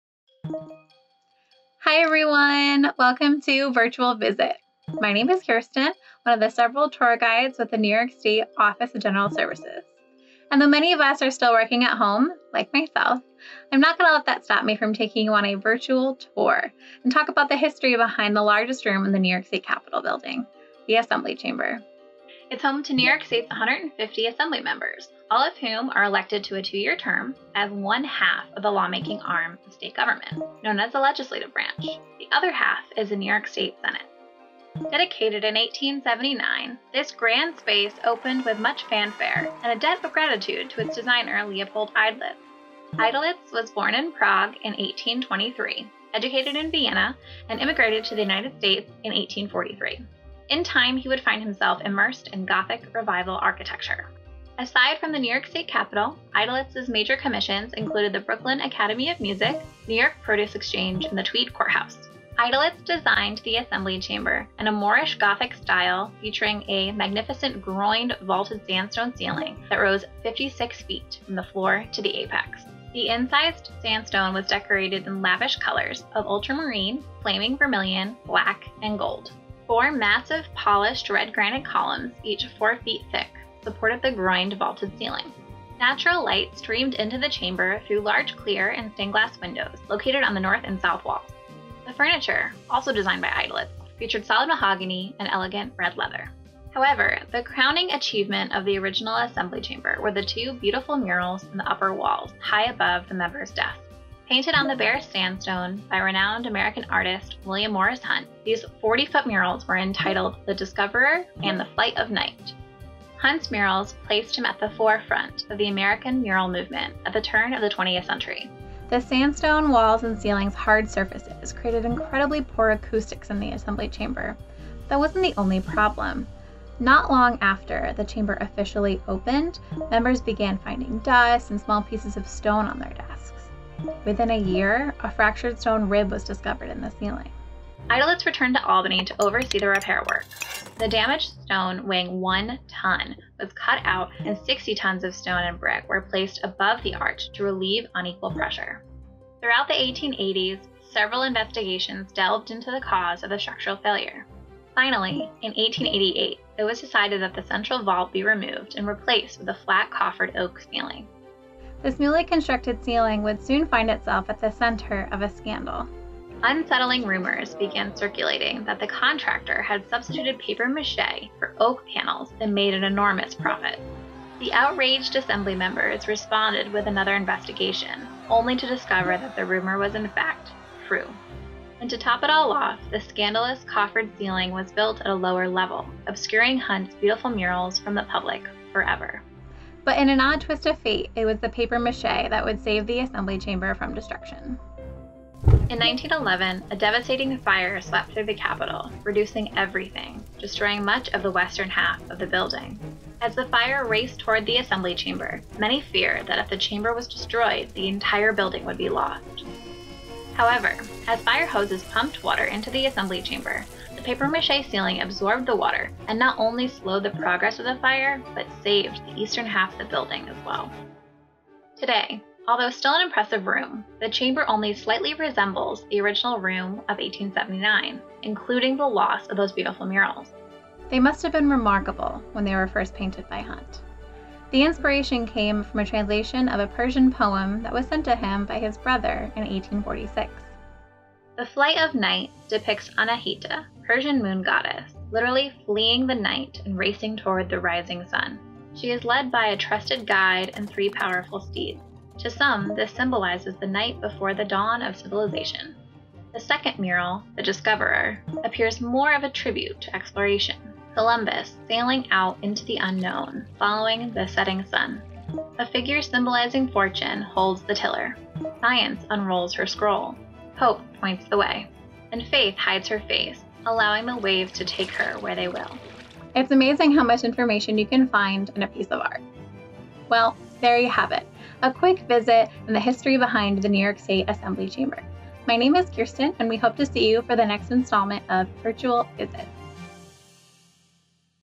Virtual Visit: NYS Assembly Chamber
Explore the architectural and artistic masterpiece that is the New York State Assembly Chamber.